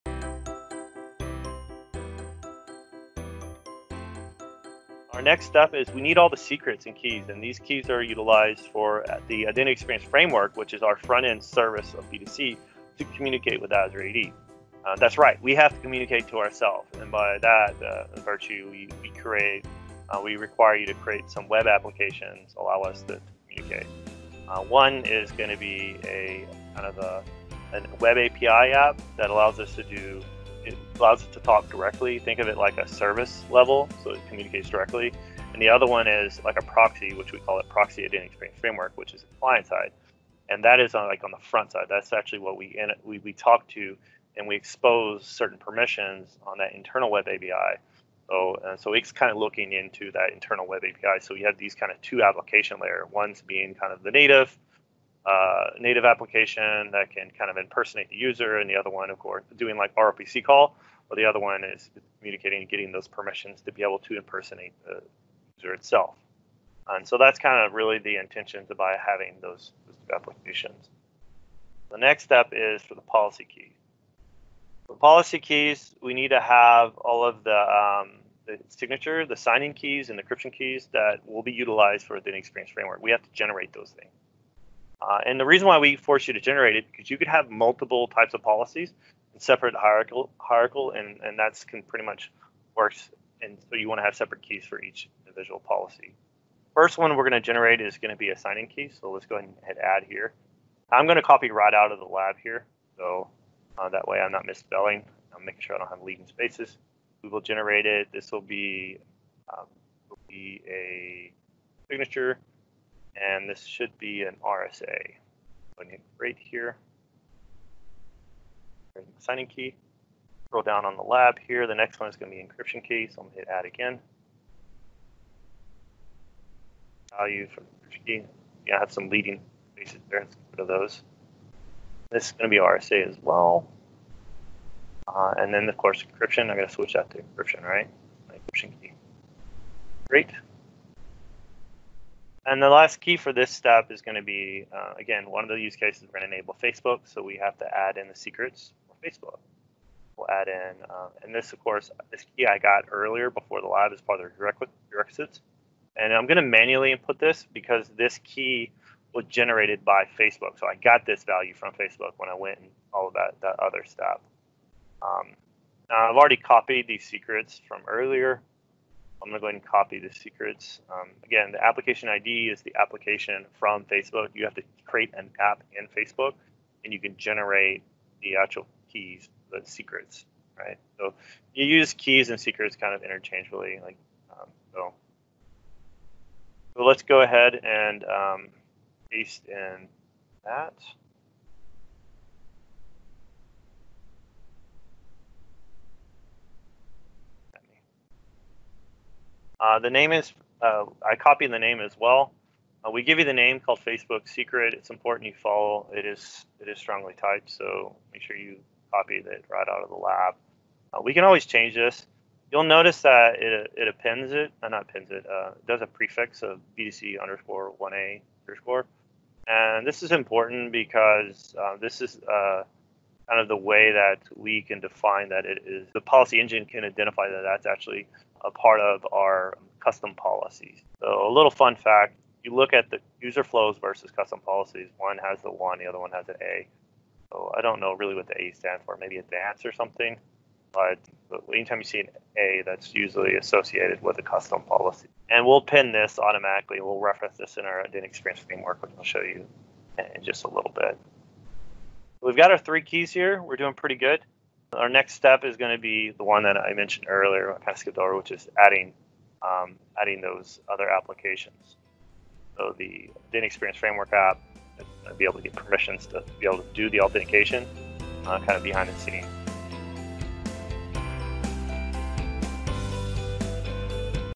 4 of 14 Create Keys/Secrets for IEF and ProxyIEF applications
To follow along with the videos with a lab, navigate and download the Word document

This is the 4th in a 14 video series. This video will walk you through how to configure the keys/secrets that are used for both the core application required to configure custom policies:
1. IdentityExperienceFramework
2. ProxyIdentityExperienceFramework

Keys or keysets... are used to put private values stored in Azure AD B2C. Behind the scene it is stored in Azure Key Vault.  In the Identity Experience Framework (IEF) references the key names and utilizes when running. When you are doing custom policy, you need to reference to these secrets. You wouldn't want to store your passwords in clear text right? That's why they are stored in the service instead of hard coding them into your XML.